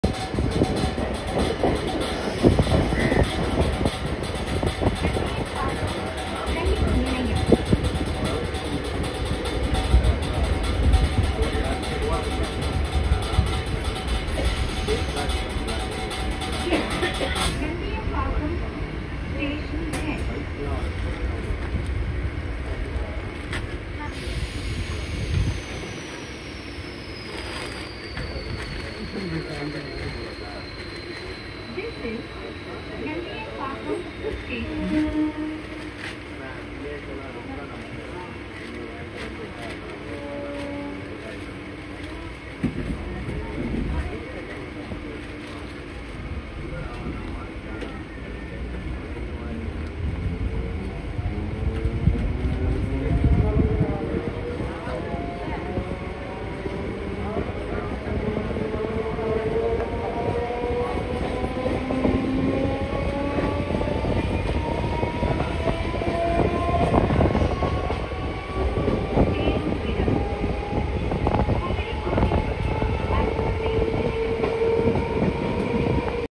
Nandiambakkam | 4K | NPKM | நந்தியம்பாக்கம் | Southern Railway | Chennai
Passing through Nandiambakkam Railway Station.

Nandiambakkam railway station is one of the railway station of the Chennai Central–Gummidipoondi section of the Chennai Suburban Railway Network.  It serves the neighbourhood of Nandiambakkam, a suburb of Chennai.